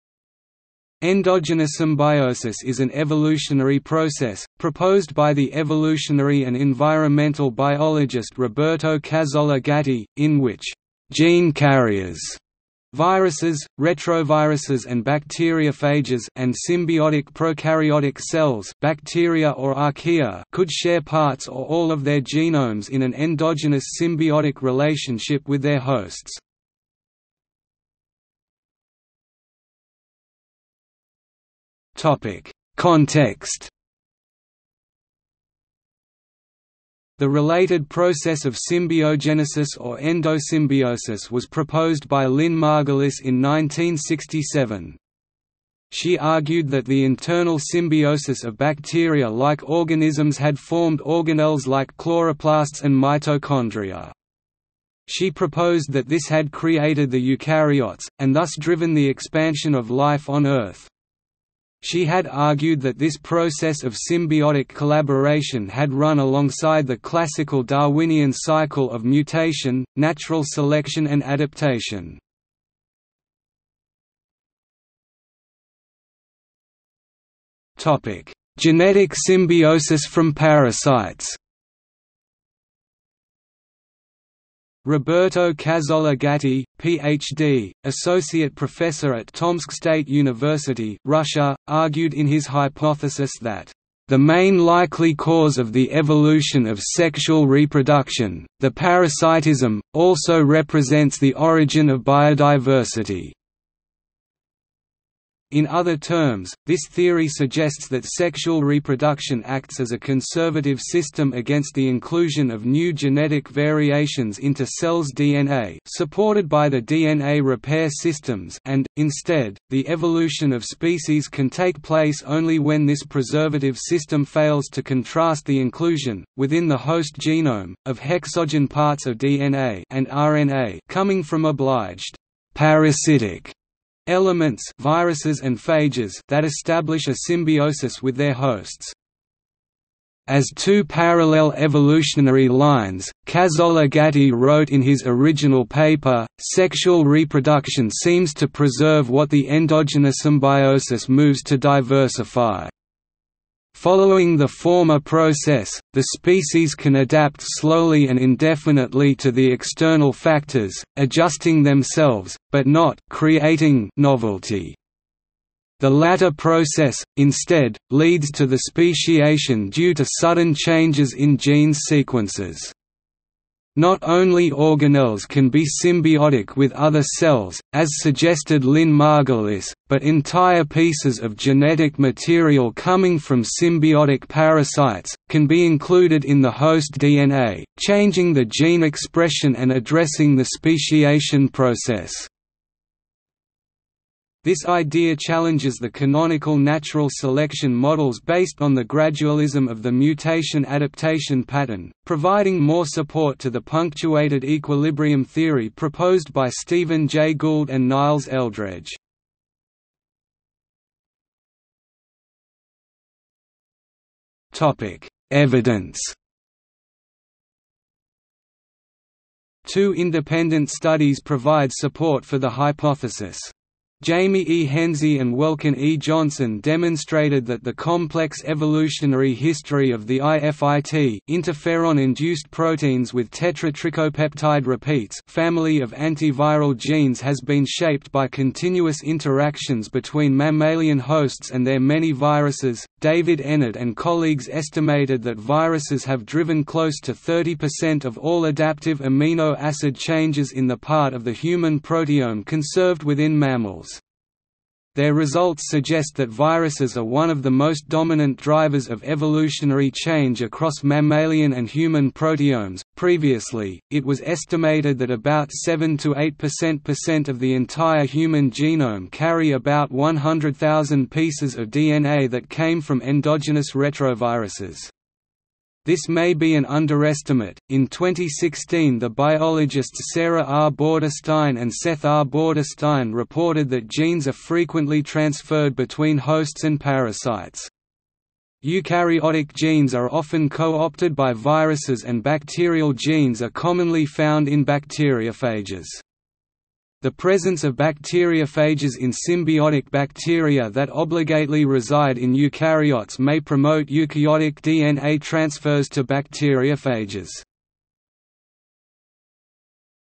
Endogenosymbiosis | Wikipedia audio article
00:00:28 1 Context
00:01:14 2 Genetic symbiosis from parasites
00:03:31 3 Evidence

- Socrates

SUMMARY
Endogenosymbiosis is an evolutionary process, proposed by the evolutionary and environmental biologist Roberto Cazzolla Gatti, in which "gene carriers" (viruses, retroviruses and bacteriophages) and symbiotic prokaryotic cells (bacteria or archaea) could share parts or all of their genomes in an endogenous symbiotic relationship with their hosts.